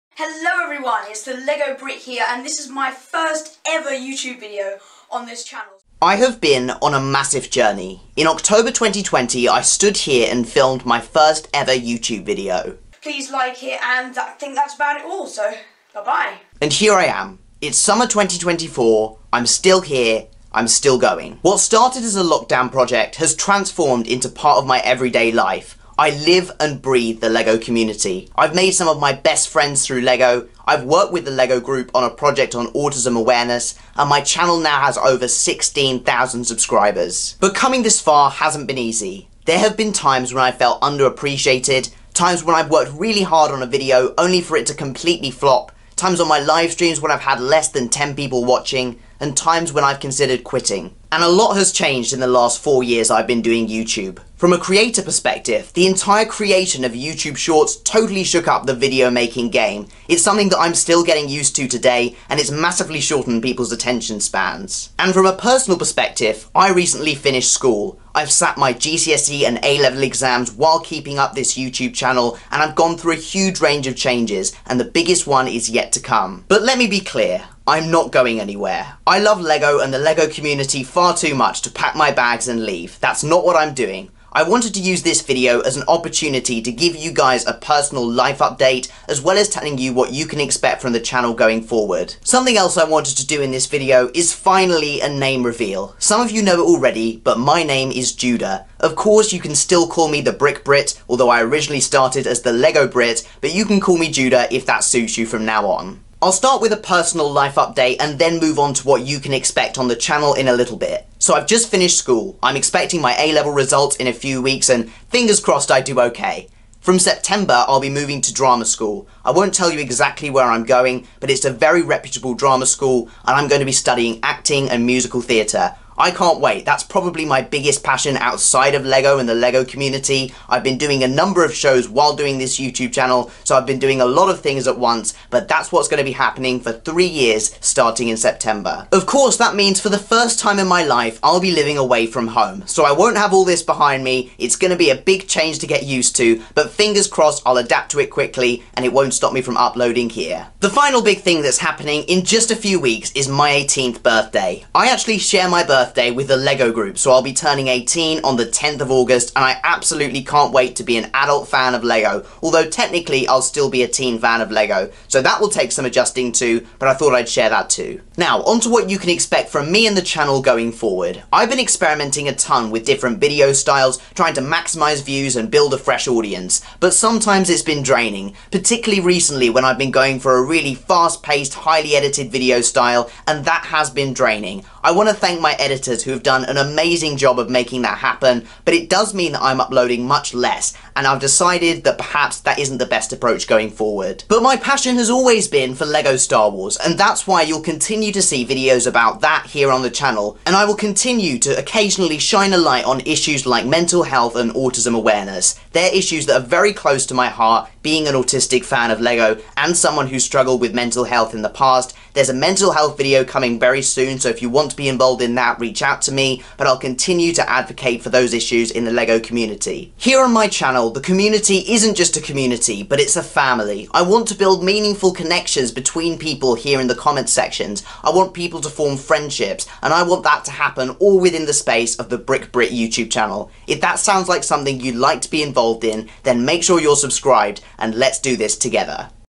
I Need To Be Honest.
It's time to examine the future of The Brick Brit YouTube channel.